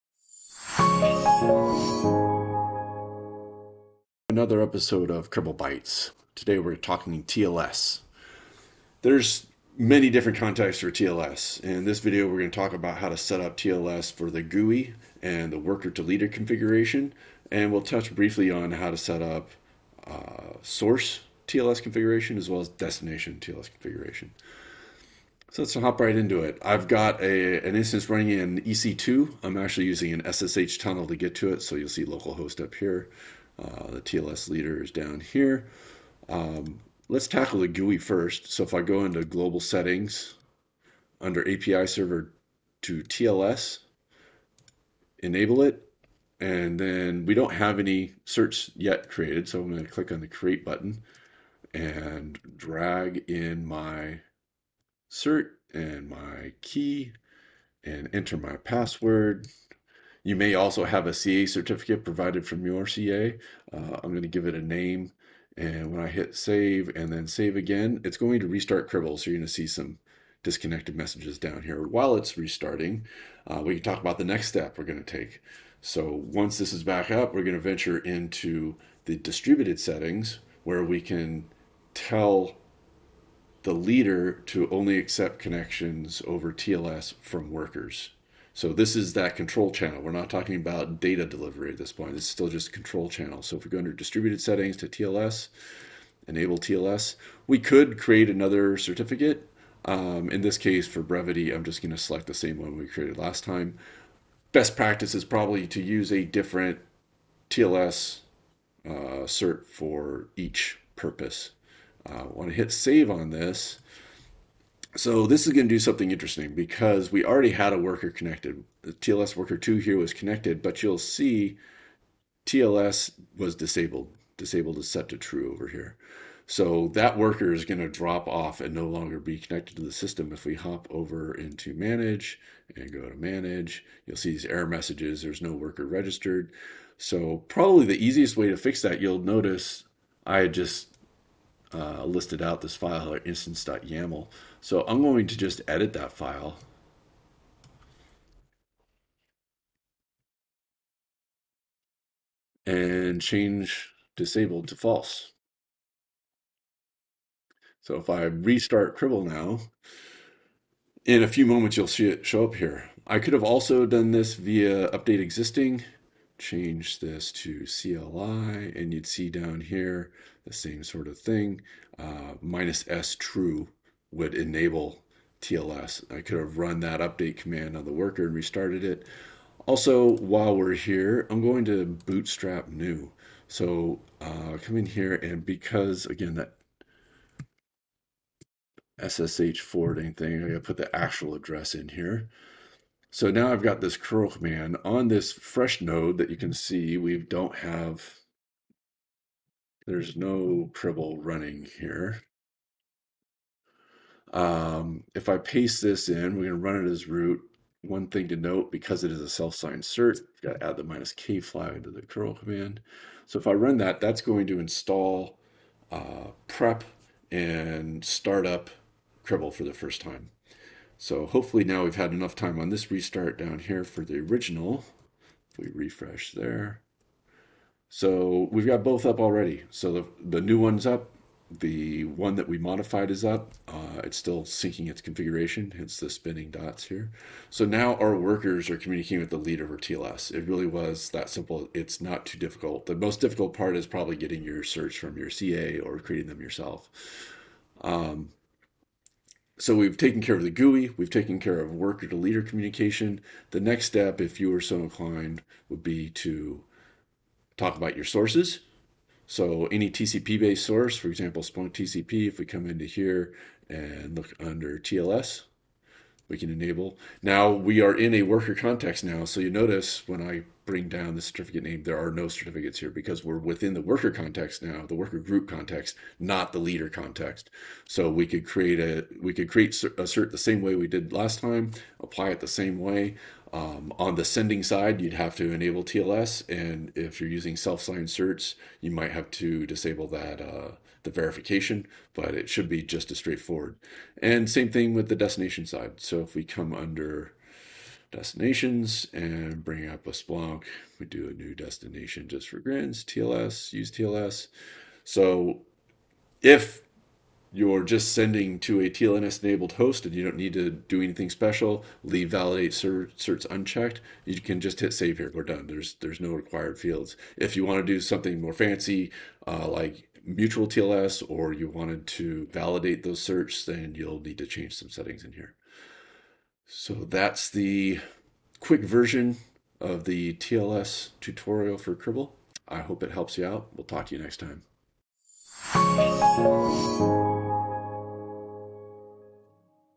Enabling TLS on a Cribl Leader for both GUI and Worker to Leader communications
In this video, we demonstrate how to enable TLS on a Cribl Leader for both GUI and Worker to Leader communications.

## Follow Cribl